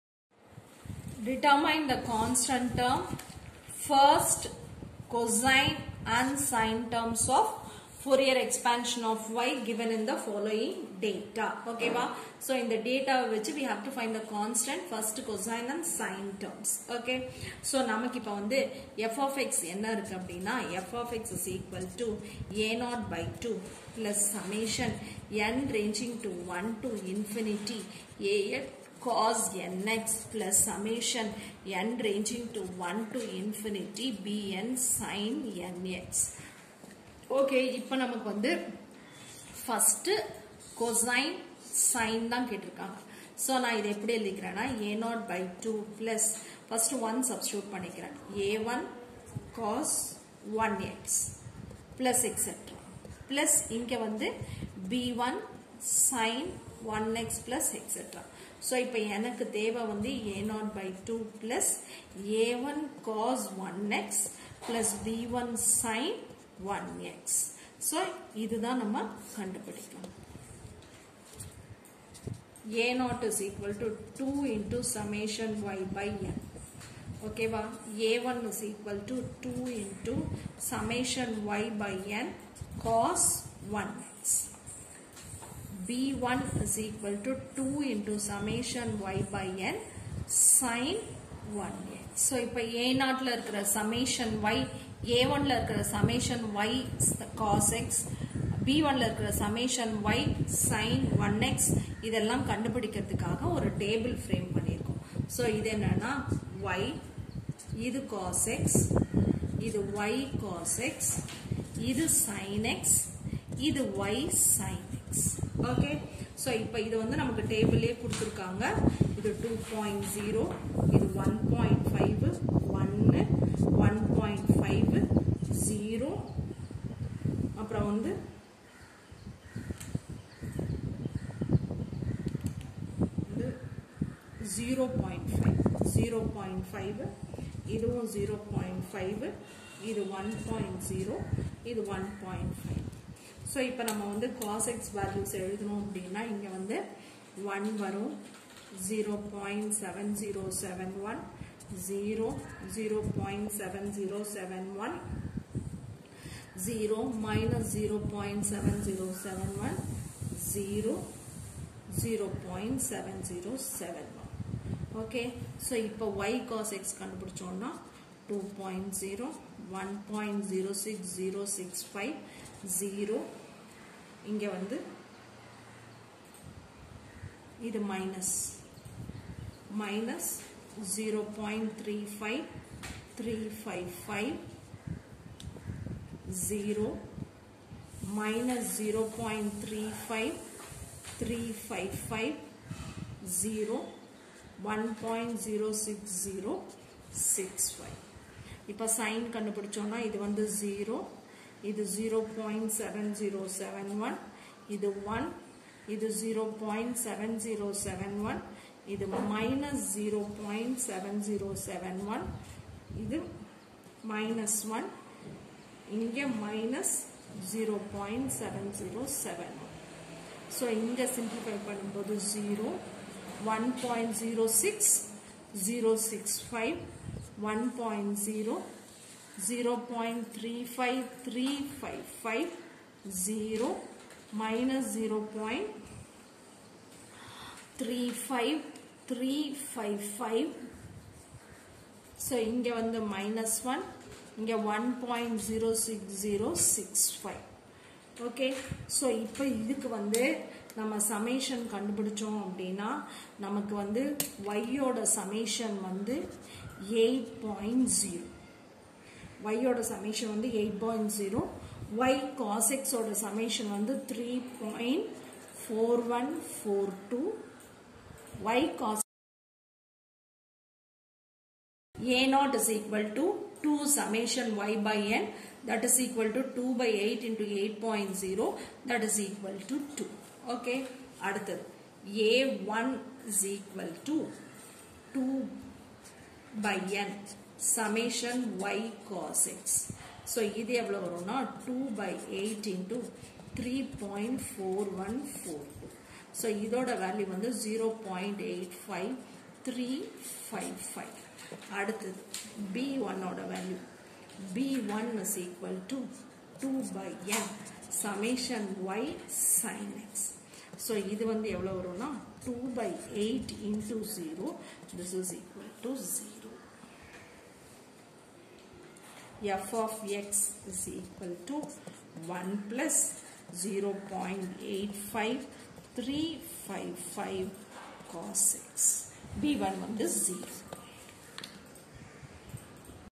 FOURIER: Obtain the constant first coefficient of cosine and sine terms of y given in the table
f(x) =x(2Π-x) x= 0 to 2Π Show that f(x) = 2 Π^2/3 - 4 = cos x + (cos 2x)/2^2 + (cos 3x)/3^2 + ... Deduce Π^2/8 = 1 - 1/3^2 + 1/5^2
Harmonic Obtain the constant first coefficients of cosine and sine terms of y given in the table
Harmonic Obtain the direct current path as 0.75A and calculate the amplitude of the first harmonic
Find the Fourier series of f(x)= x in (x = 0 to 3) / (6-x) in (x = 3 to 6)
FOURIER: Obtain the constant first coefficients of cosine and sine terms of y given in the table